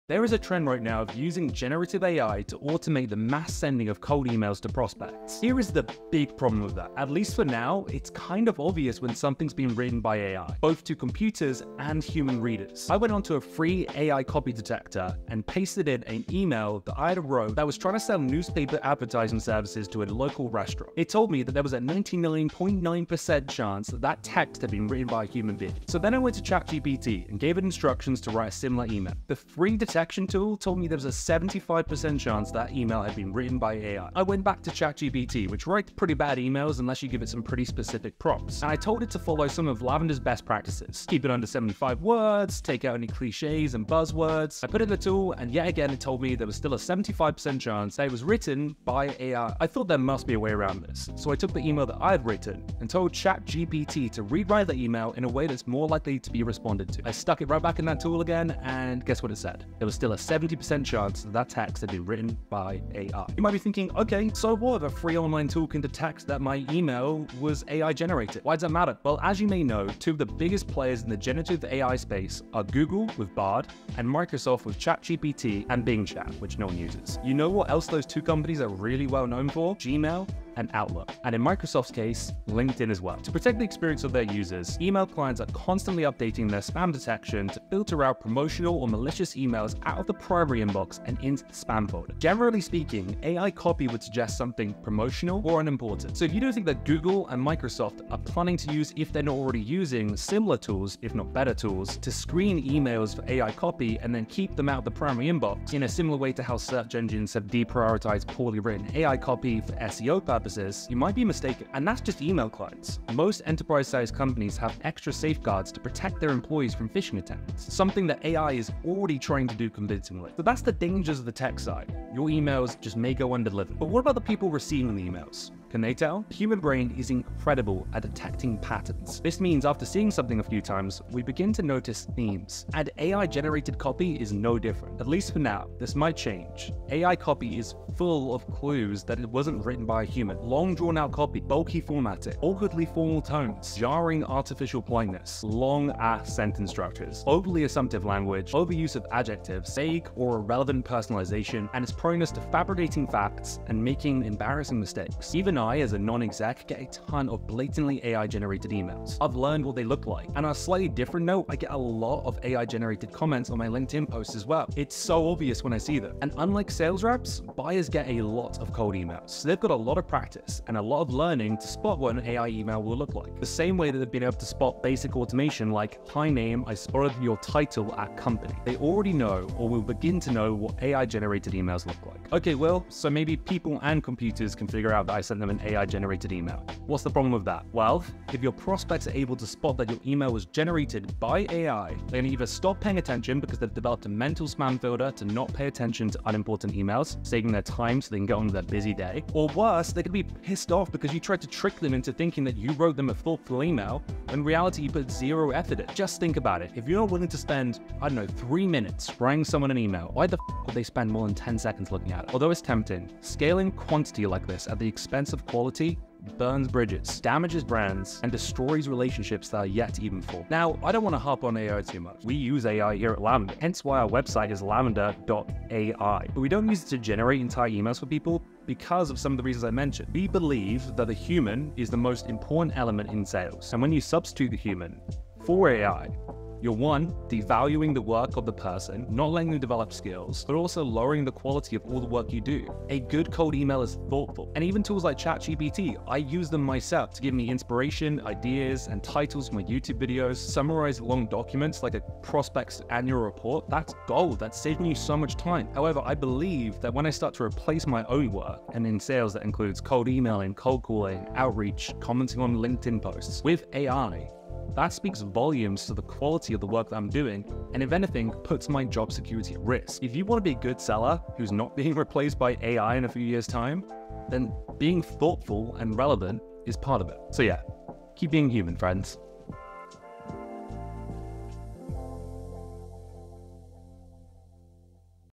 It’s Obvious AI Wrote Your Email
Using AI like ChatGPT to write cold emails has become a trend in 2023. Dozens of videos and tools promising to help you "personalize at scale" have been popping up.

In this video we take a look at some of the potential shortcomings of letting AI tools like bard, ChatGPT, and the many tools that utilize their API to write your cold emails.

TDLR - Don't Use ChatGPT To Write Cold Emails (at least until it gets better than you)